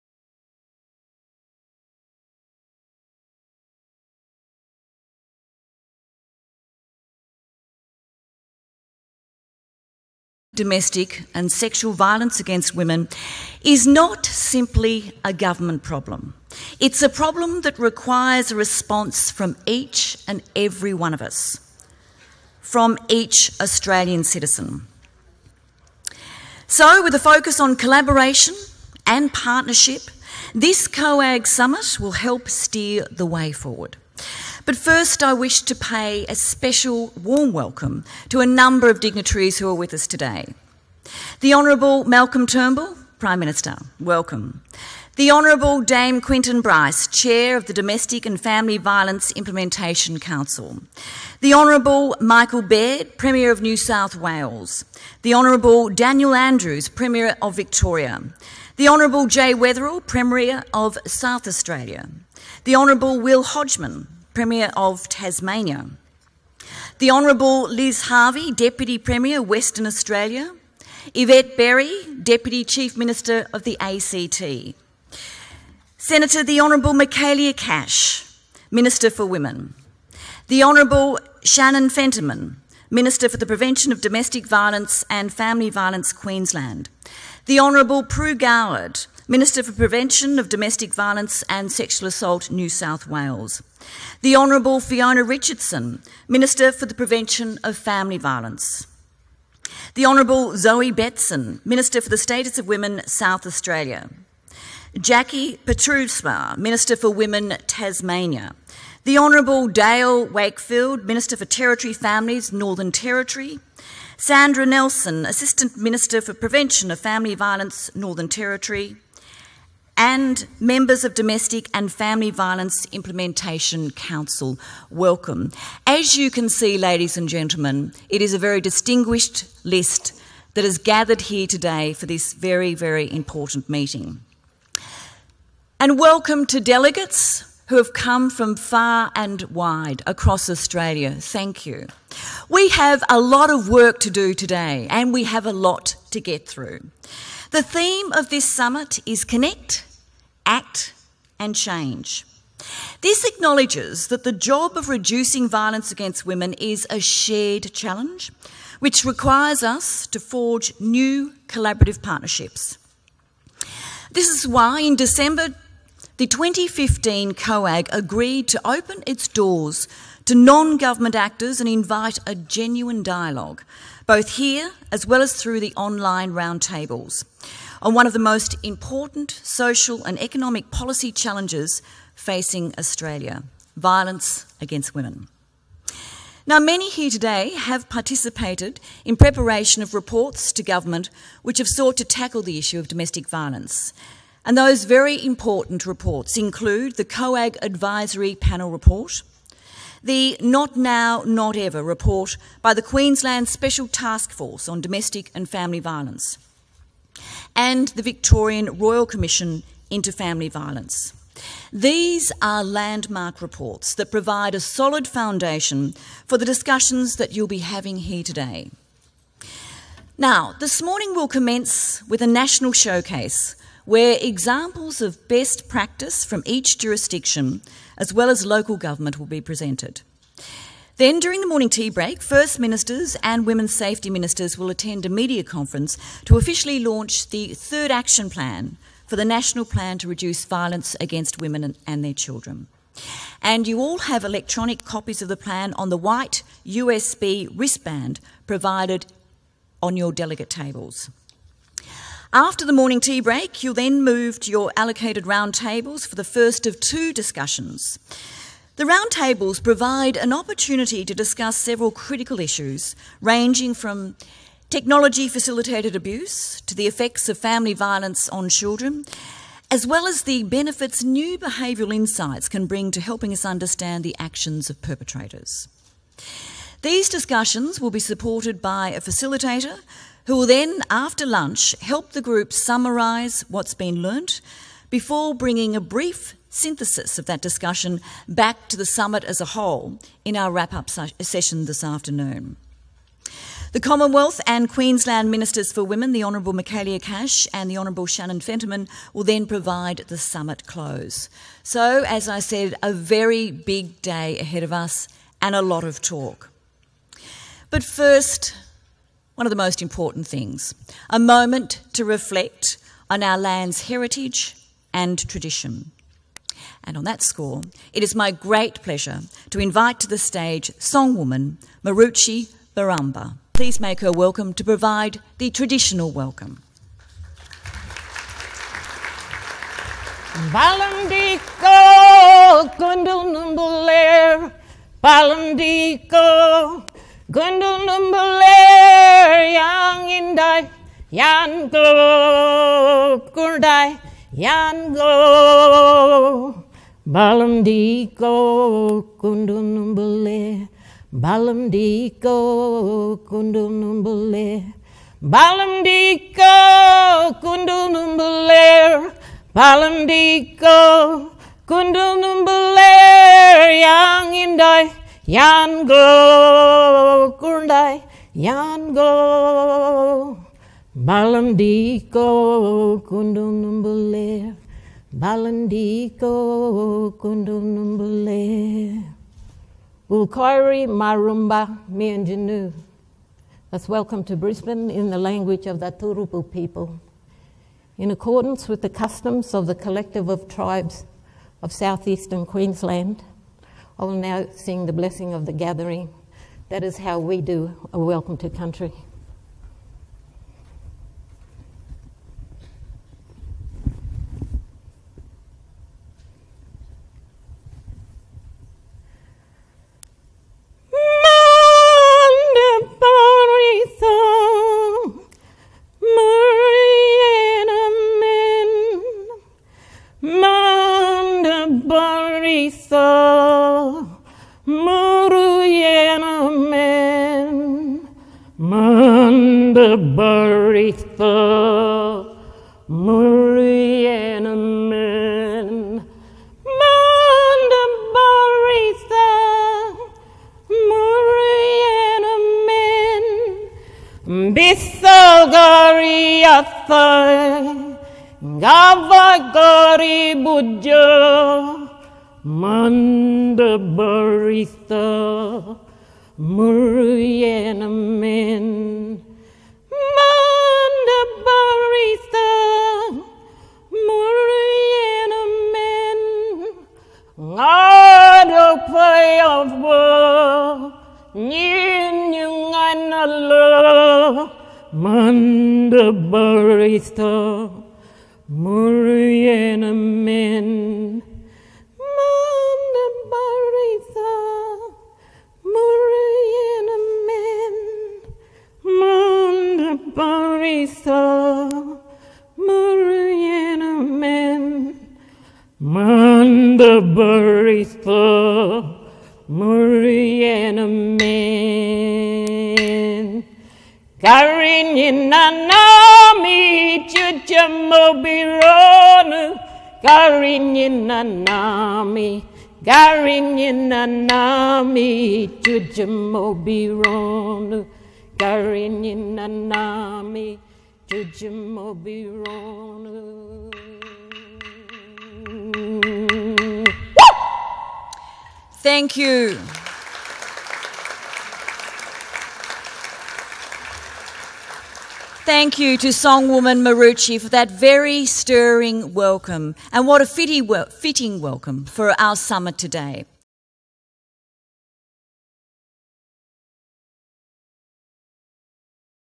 2016 COAG VAW Summit: Introduction and Welcome to Country
The introduction and Welcome to Country at the COAG National Summit on Reducing Violence Against Women and Their Children in Brisbane, Australia on 28 October 2016. The purpose of the Summit was for governments to review progress and profile best practice to make Australia safer for women and their children.